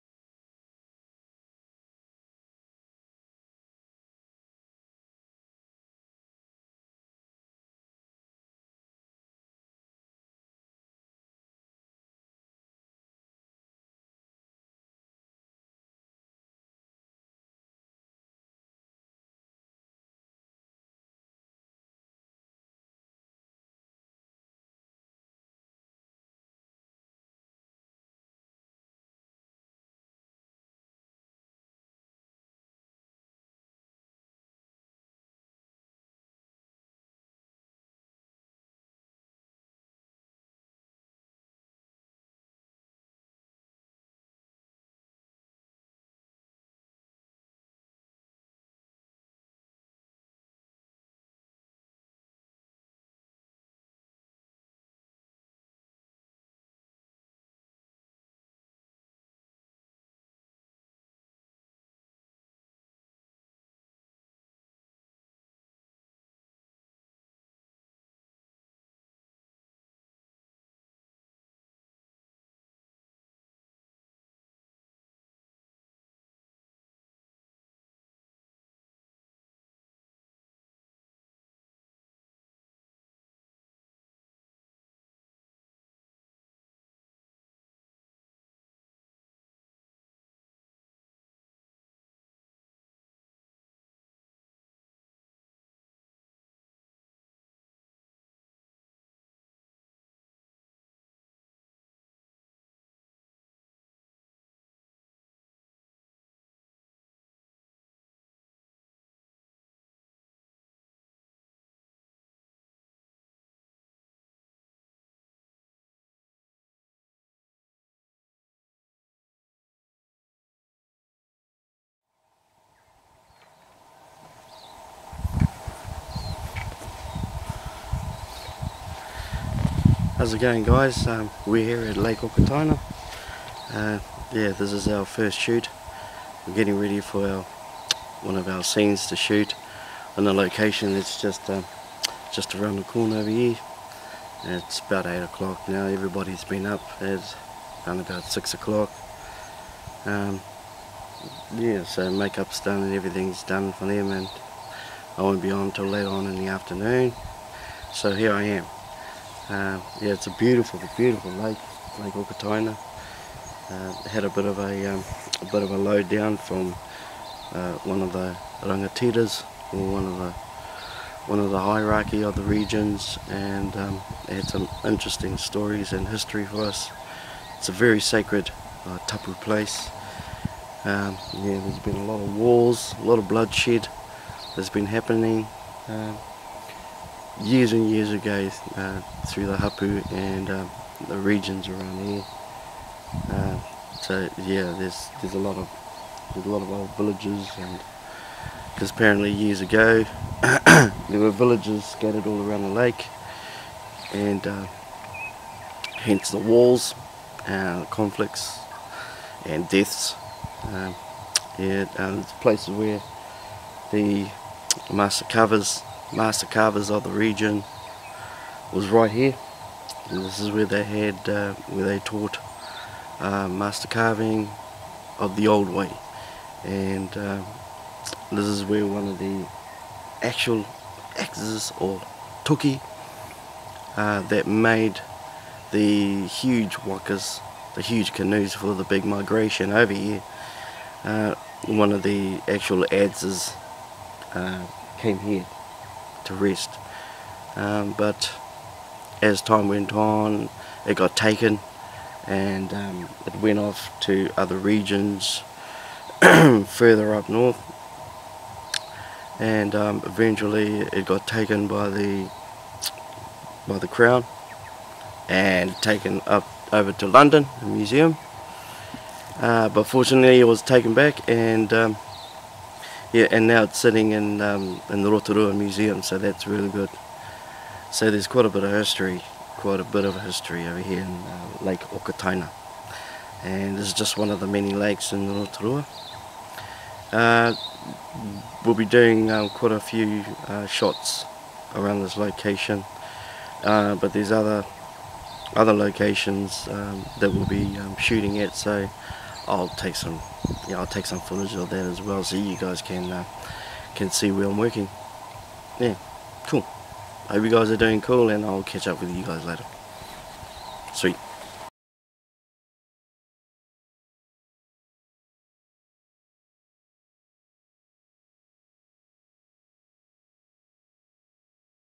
Everisingsun - On Film Location @ Lake Okataina, Rotorua, NZ - 16th March 2012.mpg
Everisingsun - Solo Artist - On location at Lake Okataina, Rotorua, New Zealand for his third film yet to be released.